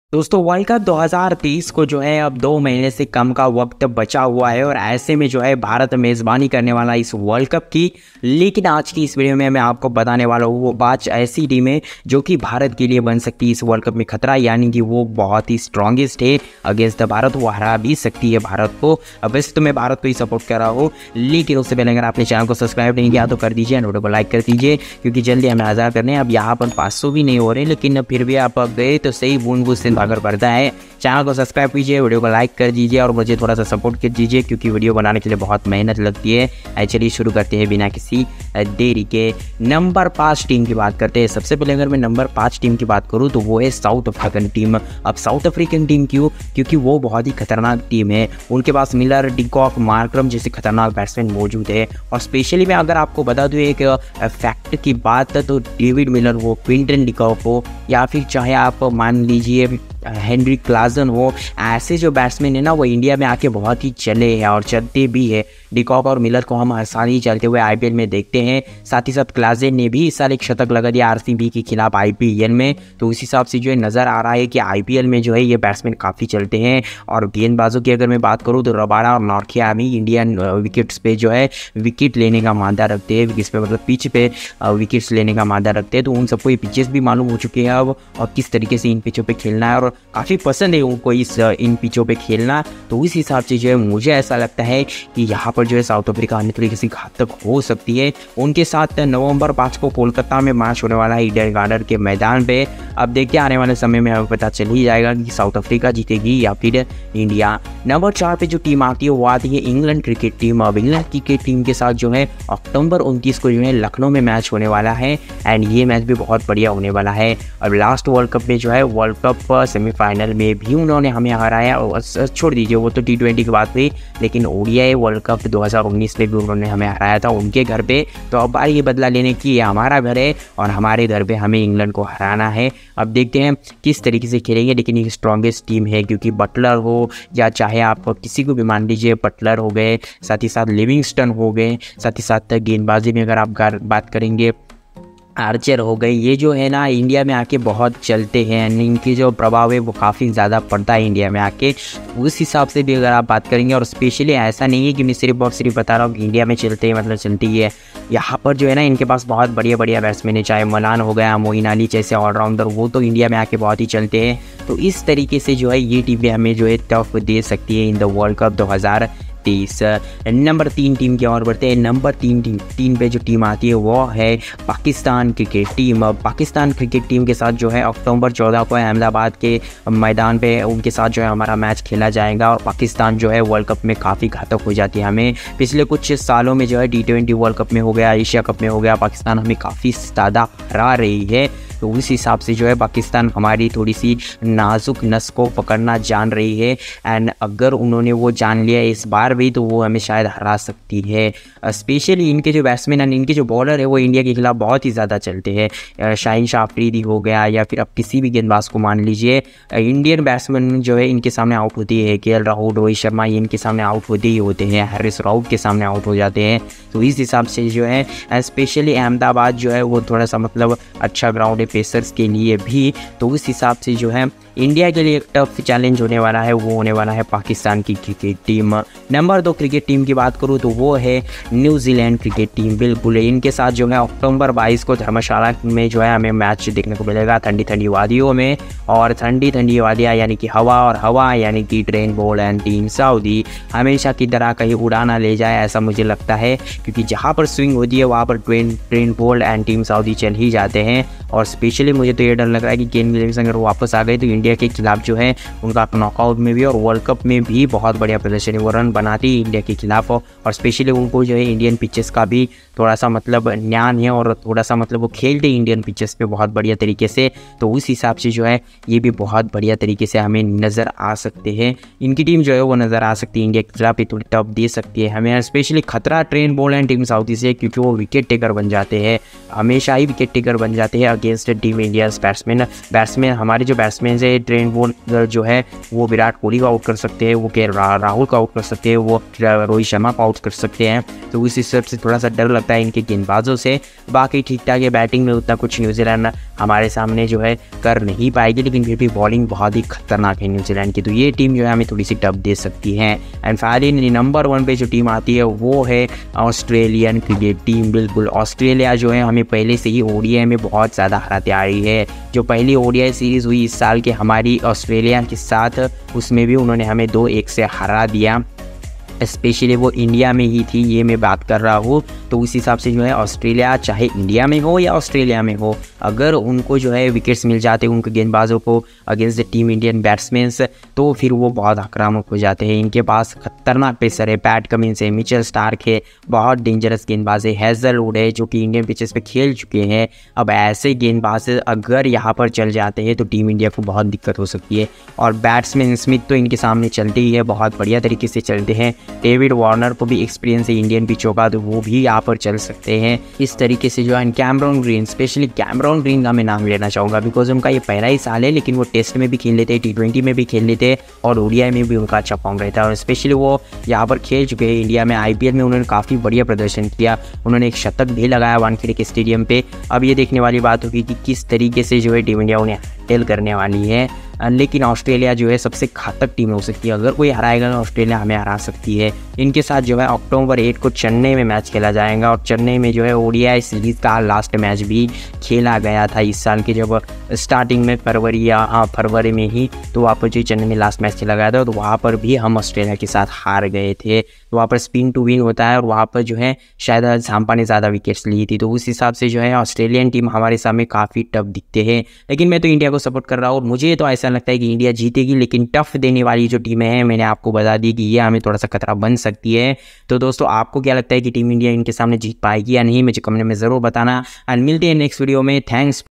Top 5 Teams Against Team Indian In World Cup 2023||WORLD CUP मे ये 5 TEAMS बनेंगी भारत के लिये खतरा
TOP 5 DENGOURES TEAMS AGAINST INDIA IN WORLD CUP 2023